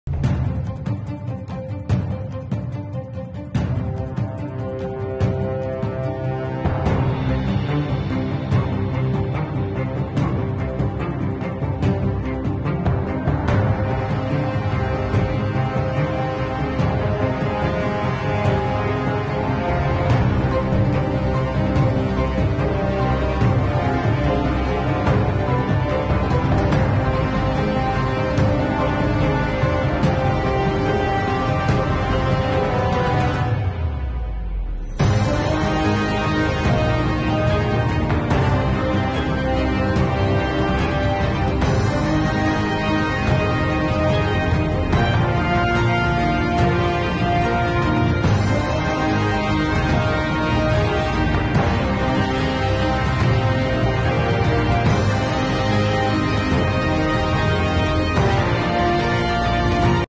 Fitness Pre-Workout promotional video: Optimum Nutrition
Here  i attempted to create an eye catching video for the pre workout that i use